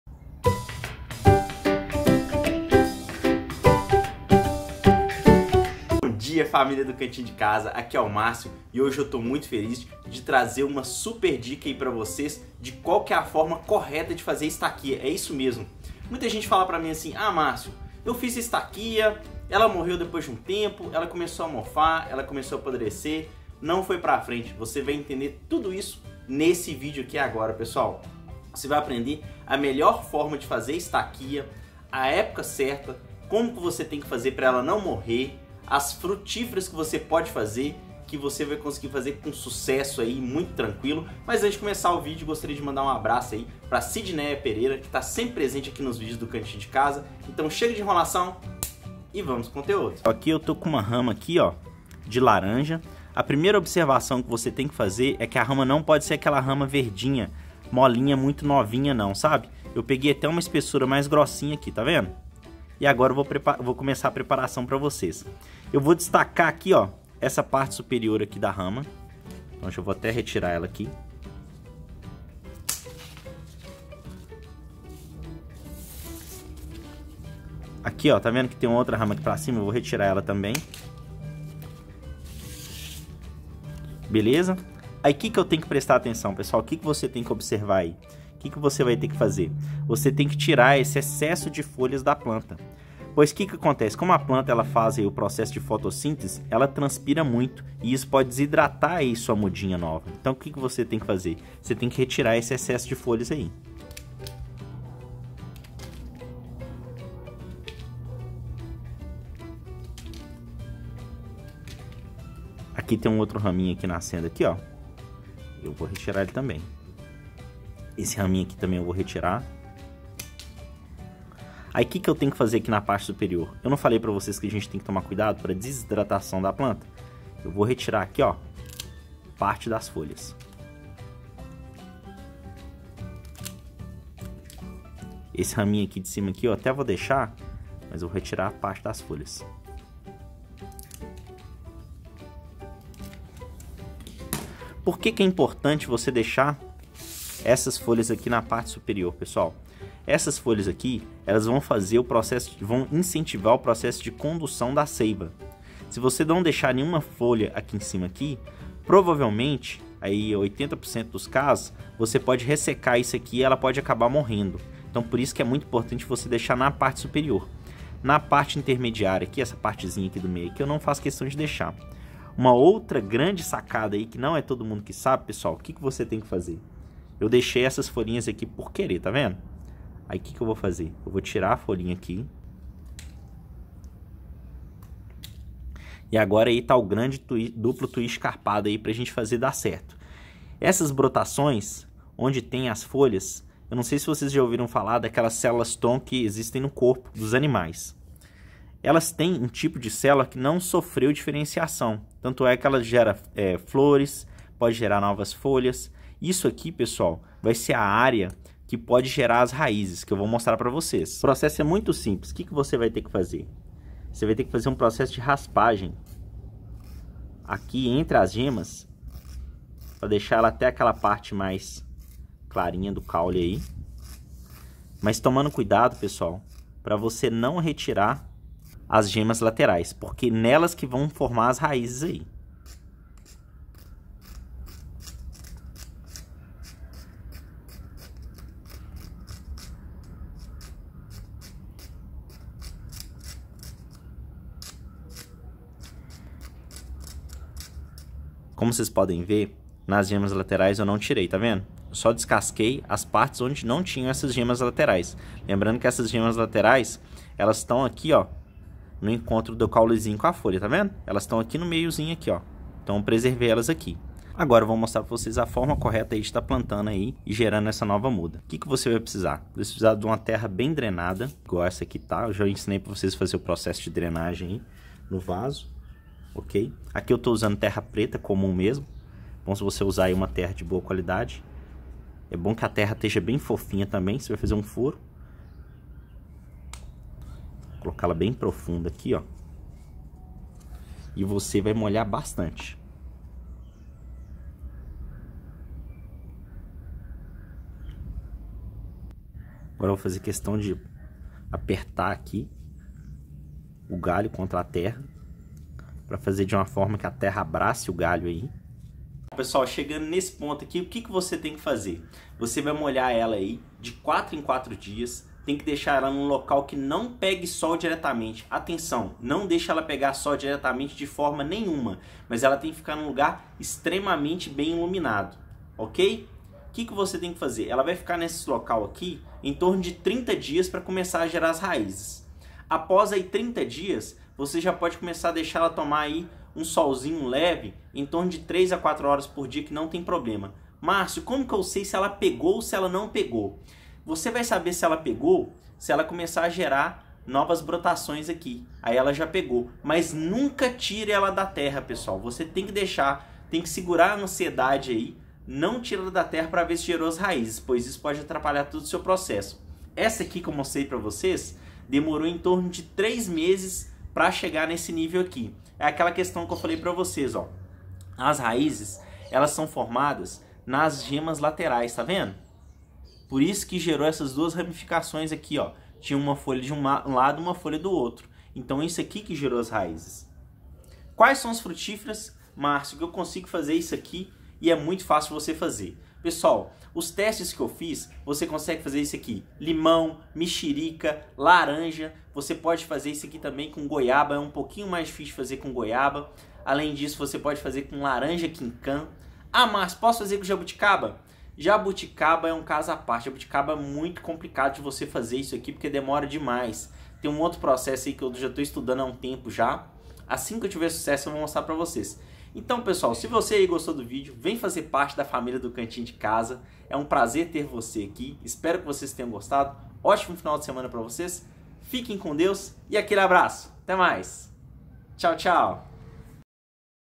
Como Fazer estaquia da forma correta! (nova muda em 30 dias)
Passo a passo para fazer estaquia de sucesso em 30 dias, saiba quais são as frutiferas mais fáceis de pegar, epoca do ano, como plantar, e como preparar a muda!